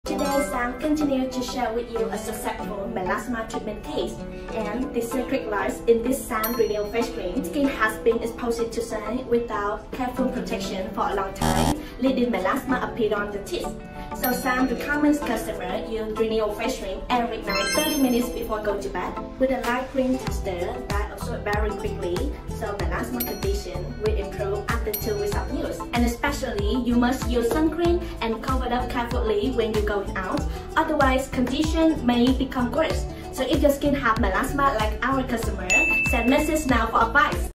Remove MELASMA spots on face from 2 WEEKS | Some successful cases from SAAM
How long do you plan to let melasma affect your confidence, 1 year, 5 years or forever? Remove melasma spots on your face from today with just 1 face cream from SAAM. Many customers have used it and achieved expected results, what about you?